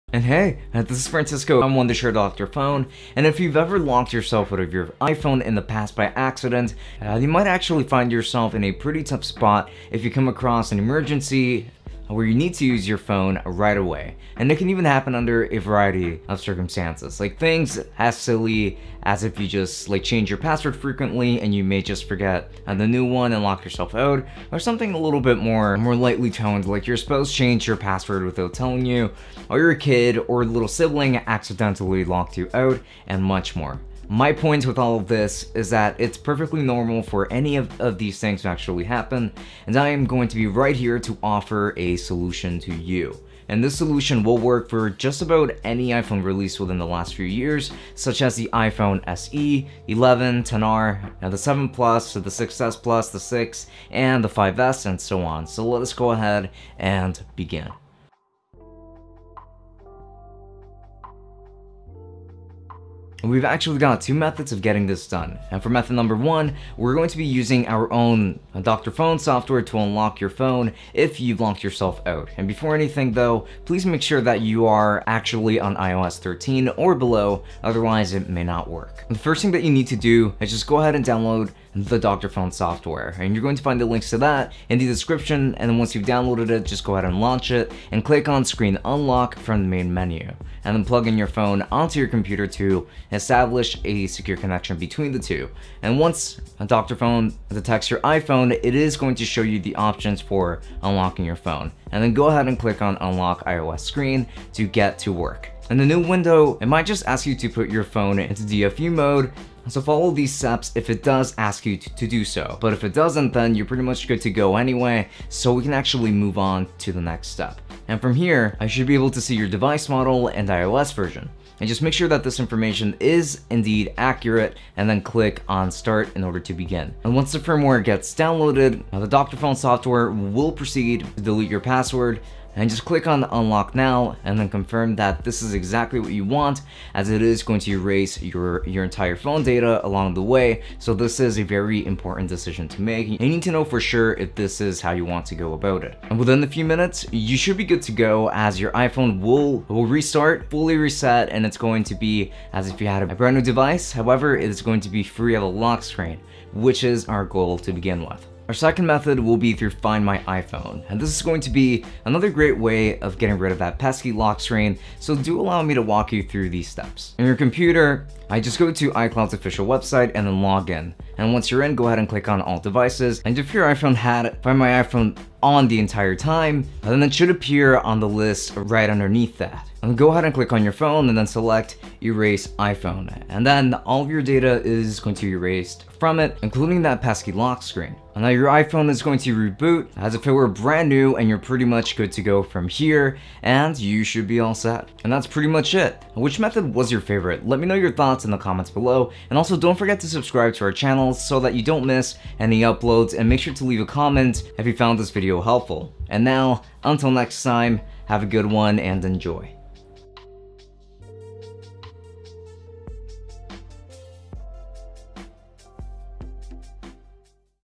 How To Unlock iPhone SE/11, XR, 7 Plus, 6S Plus, 6, and 5S
This video offers solutions for you to unlock your iPhone SE/11, XR, 7 Plus, 6S Plus, 6, and 5S

Solution 1 Unlock your iPhone by Dr.Fone
Solution 2 Unlock your iPhone by Find My iPhone

Timestamps:
01:32 Solution 1 Unlock your iPhone by Dr.Fone
Step1 Plug your iPhone
Step2 Launch Dr.Fone and select Screen Unlock
Step3 Unlock iOS screen
Step4 Select the correct iPhone model and iOS version
Step5 Once the firmware is downloaded, click Unlock Now

02:38 Solution 2 Unlock your iPhone by Find My iPhone
Step1 Go to the iCloud official website and log in
Step2 Click on Find iPhone
Step3 Click on your iPhone and click erase iPhone